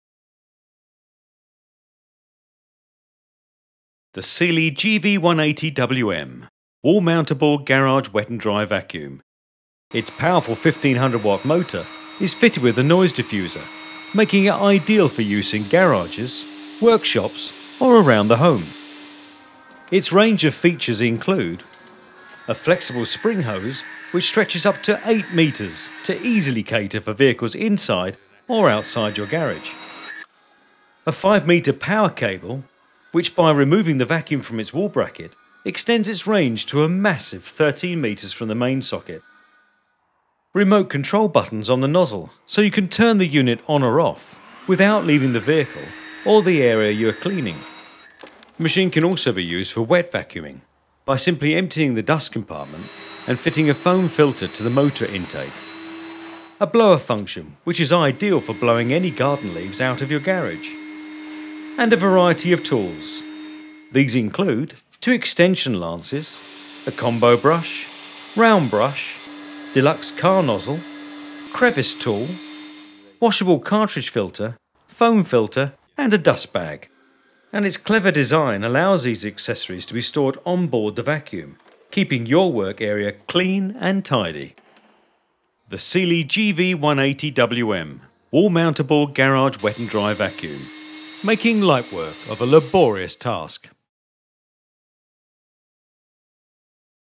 Sealey GV180WM Wet & Dry Garage Vacuum
Demonstration of this powerful, wall-mountable garage vacuum.

1500W Wall Mounting Garage Vacuum with Remote Control (GV180WM)

for more information.